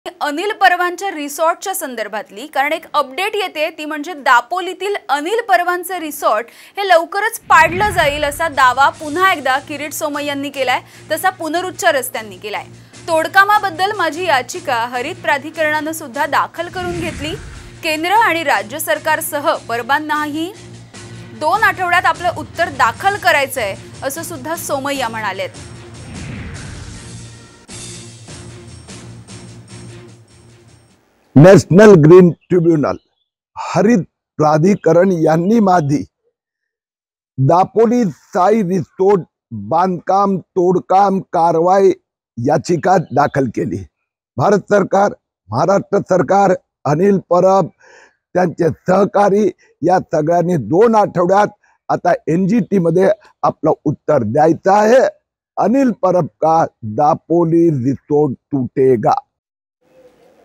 Kirit Somaiya on Anil Parab | अनिल परबांचे रिसॉर्ट पडणारचं !; किरीट सोमय्यांचा इशारा
Watch The Latest News on Kirit Somaiya on Anil Parab | अनिल परबांचे रिसॉर्ट पडणारचं !; किरीट सोमय्यांचा इशारा